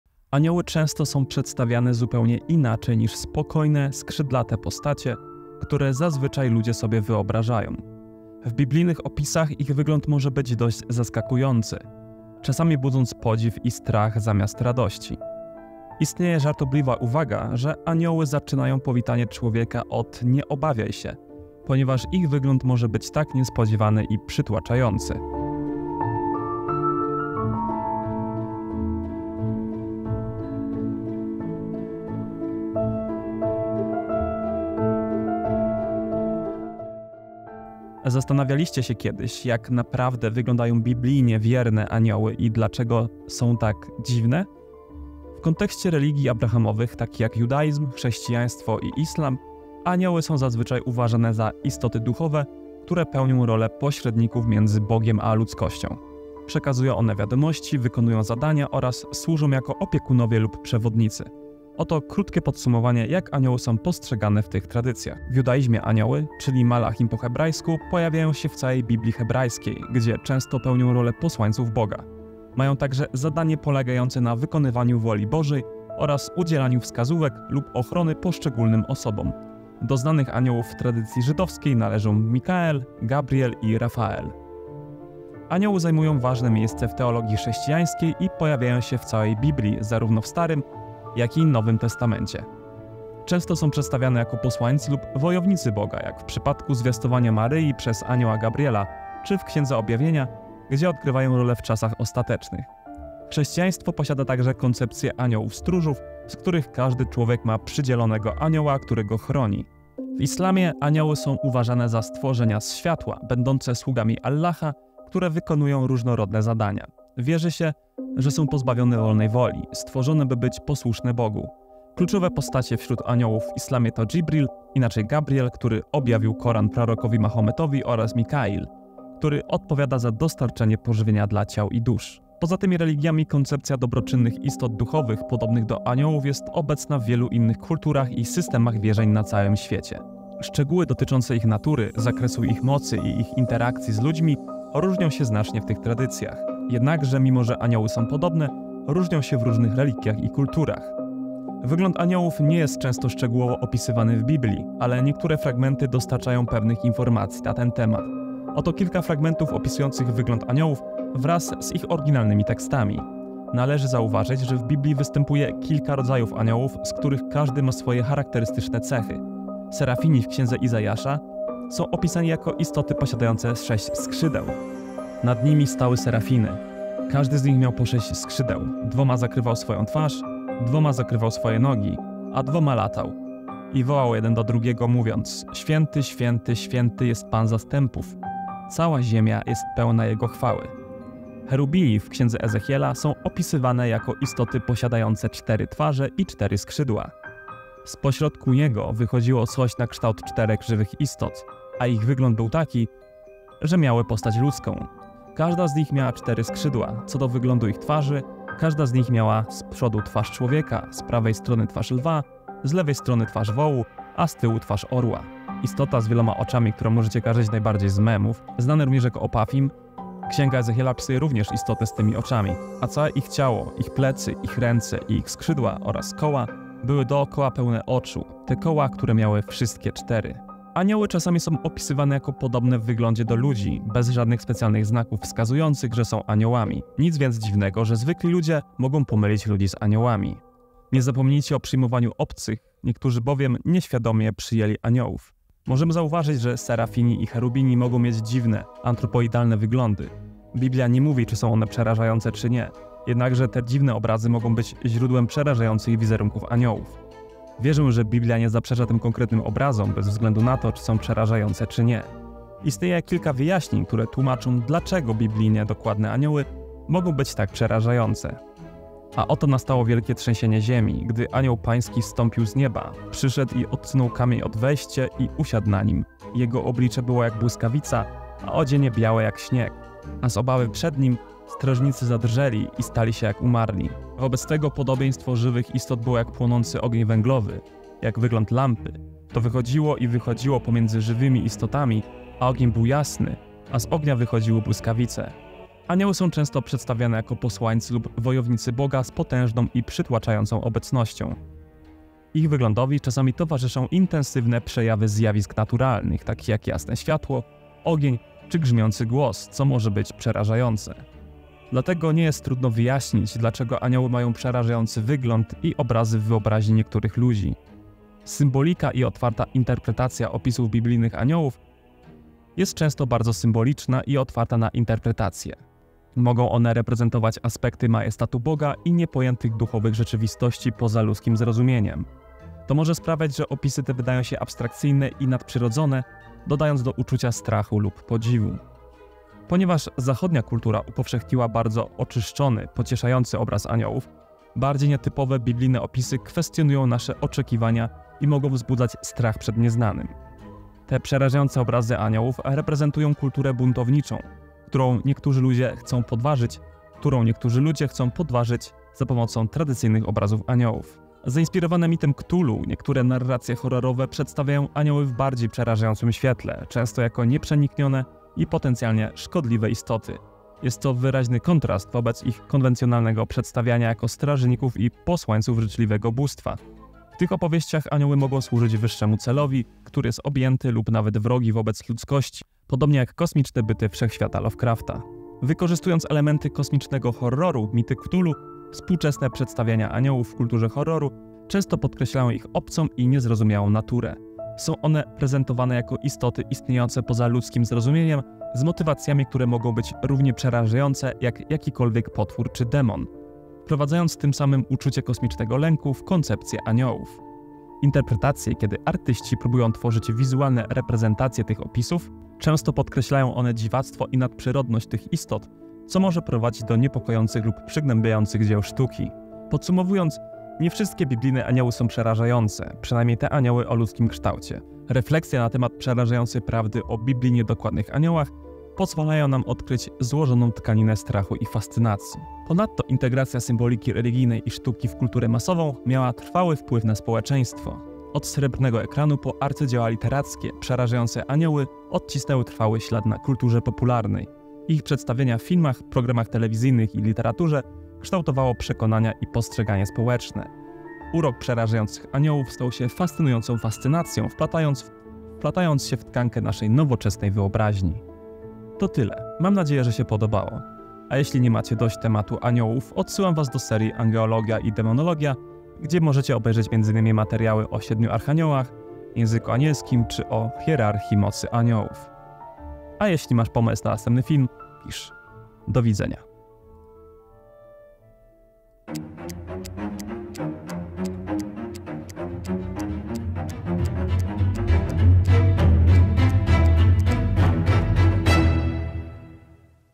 Dlaczego biblijne anioły są przerażające?
I rzekł do niej anioł: Nie bój się, Maryjo, znalazłaś bowiem łaskę u Boga". -Anioł Gabriel do Marii, Łukasz 1:30 (KJV)

Czasami anioły pojawiają się jako przerażające postacie, które znacznie różnią się od tego, co widzimy jako piękne kobiety lub dzieci z pierzastymi skrzydłami. Zazwyczaj uważa się je za anioły zgodne z Biblią.

Czy jesteście ciekawi, jak wyglądają biblijne anioły?